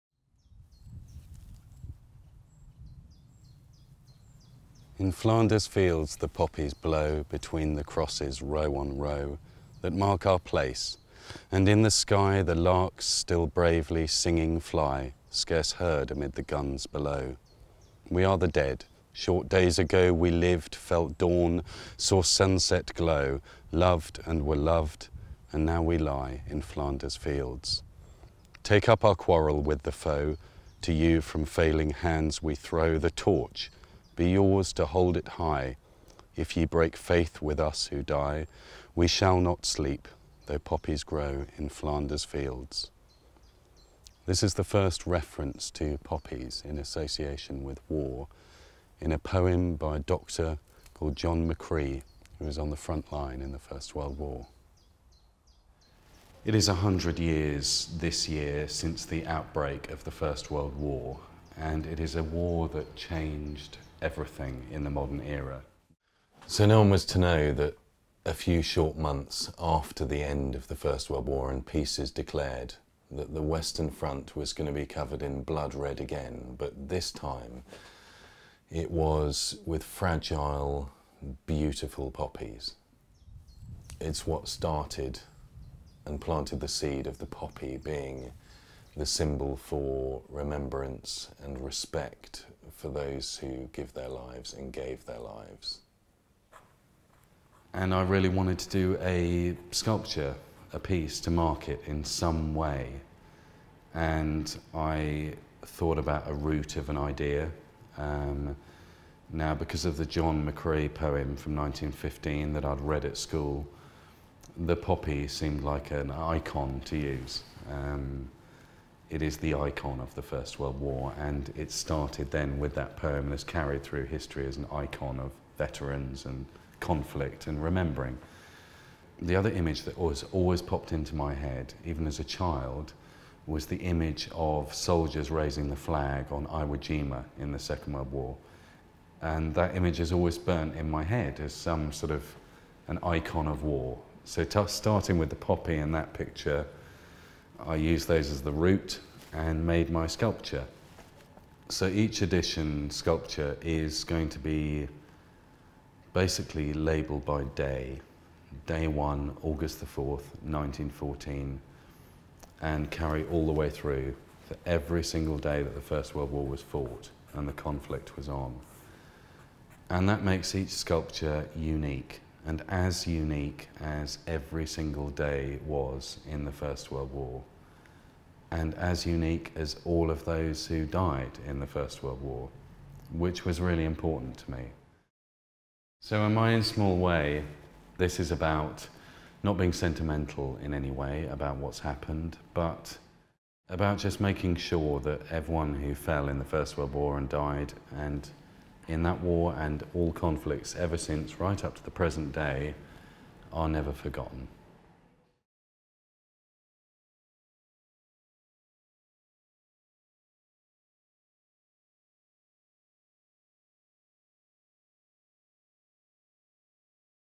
Never Forgotten by Nic Joly
British artist Nic Joly has used his mastery of miniature sculpture to mark the 100th anniversary of the outbreak of World War I in his own inimitable fashion.

This special piece has been produced in association with The Royal British Legion and aims to raise awareness and funds for war veterans and servicemen.